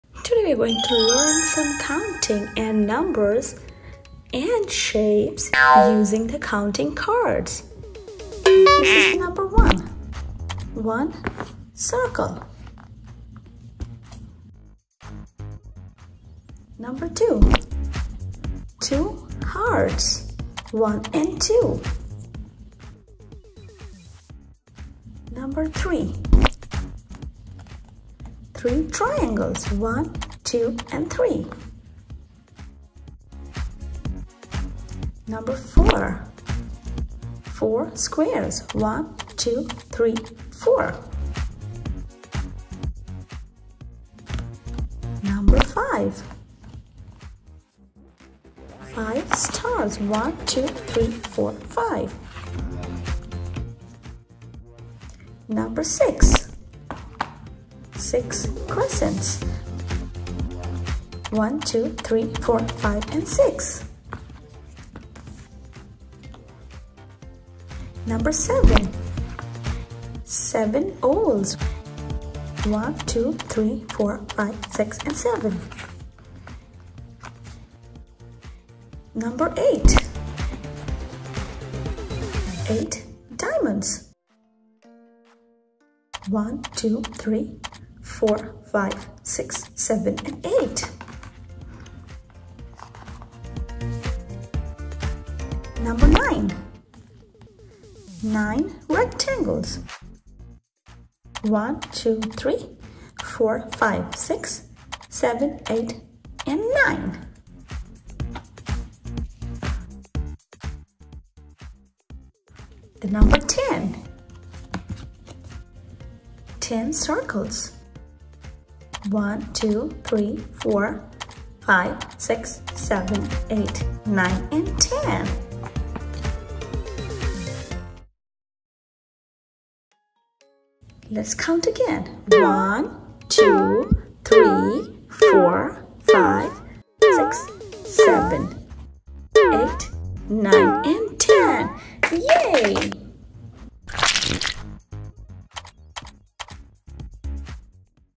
Learn Numbers, Counting and Shapes - Pre school Education - Best Learning Videos for Toddlers
"Welcome to our fun-filled educational video 'Learn Numbers, Counting and Shapes' – a perfect learning resource for toddlers and preschoolers! 🎈🔢🔷

This engaging video is designed to introduce your little ones to the basics of numbers and shapes playfully and interactively. Join us as we explore the world of early mathematics and geometry, making learning a joyful and exciting experience for your child.

🌟 What You'll Discover:

- Easy-to-understand lessons on numbers and counting.
- An introduction to basic shapes like circles, squares, and triangles.
- Colorful animations and characters that captivate and educate.
- Fun activities and examples to reinforce learning.
- Ideal for toddlers and preschoolers, enhancing their cognitive skills and preparing them for school.
- Our video is more than just an educational tool; it's an adventure into the basics of math and geometry that your kids will love!